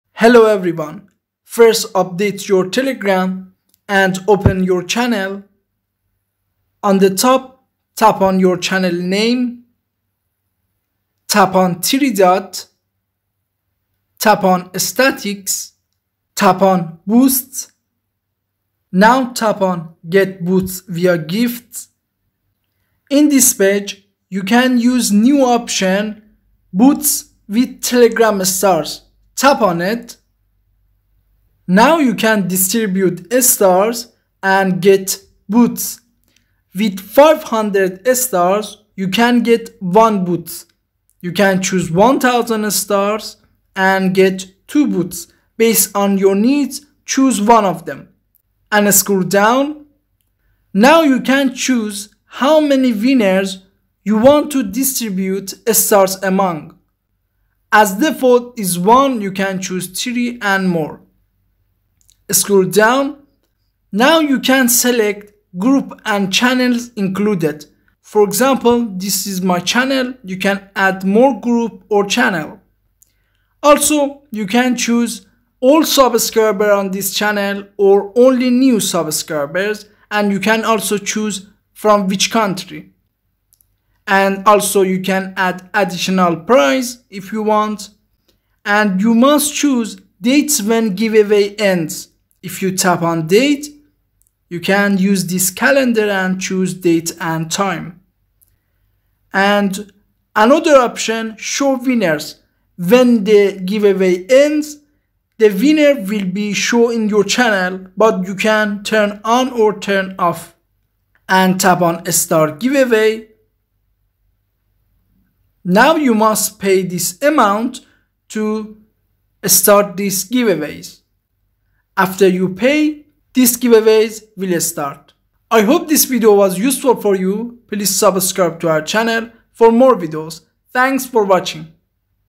how to use Star Giveaways in telegram - boosts with stars
in this video we learn how to use Star Giveaways in telegram - boosts with stars. In the new update, Telegram channel owners can boost their channel through Star Giveaways or stars and gain access to new features. In this video, I showed you the steps to use Star Giveaways.